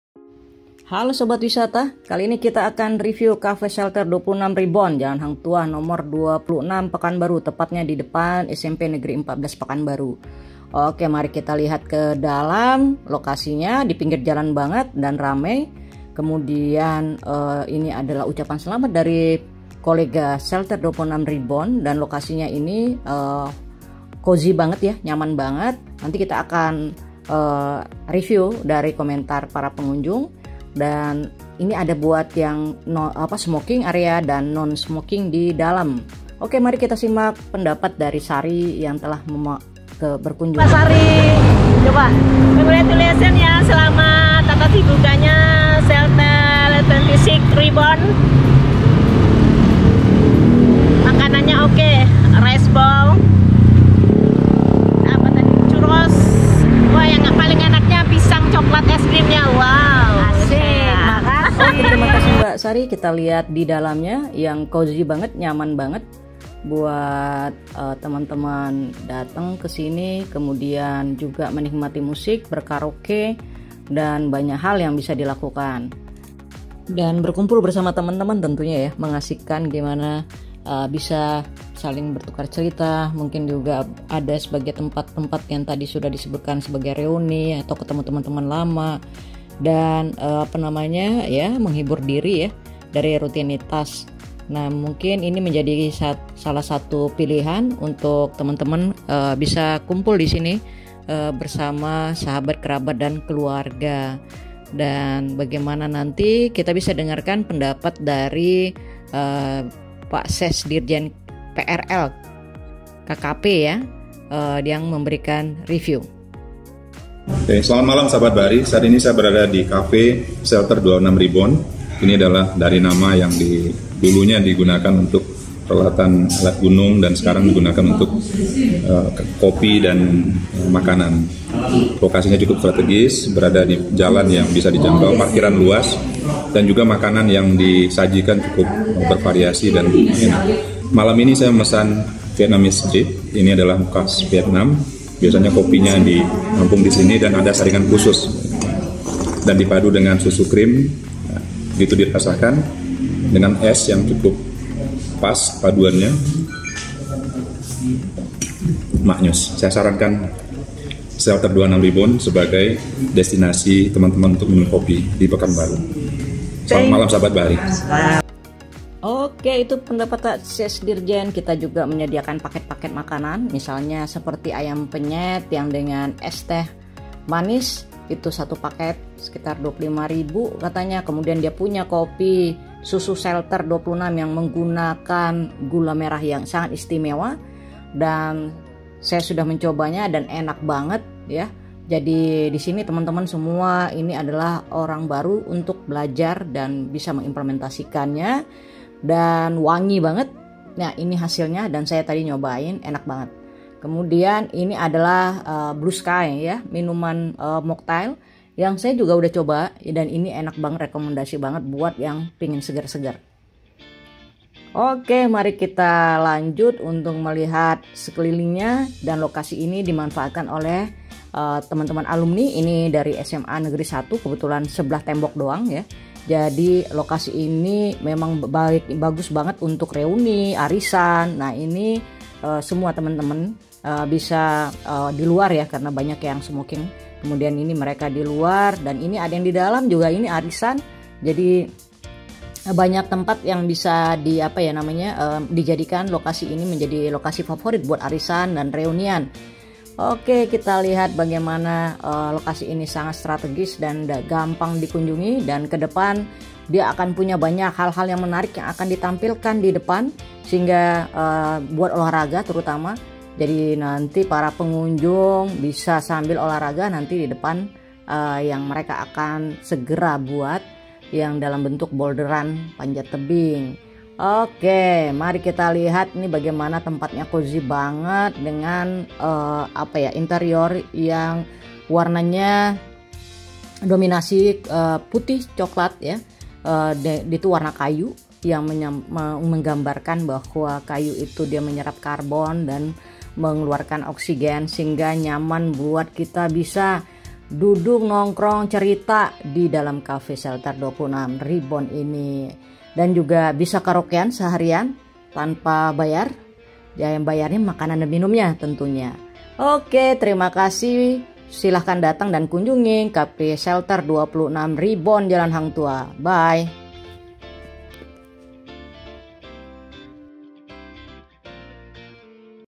Shelter 26 Reborn Tempat Asyik Untuk Nongkrong di Pekanbaru
Shelter 26 Reborn adalah Cafe baru di buka tanggal 26 Januari 2022, yang sebelumnya merupakan toko outdoor kemudian di pugar dan didisain untuk tempat asyik untuk nongkrong, belajar, dan meet up bareng teman dan kolega, dilengkapi wifi yang super kencang banget, tempatnya sejuk untuk belajar, ataupun meet up, memiliki parkiran yang luas, dan ruang terbuka bagi yang ngerokok. So, cocok banget untuk segala umur nongkrong di tempat ini.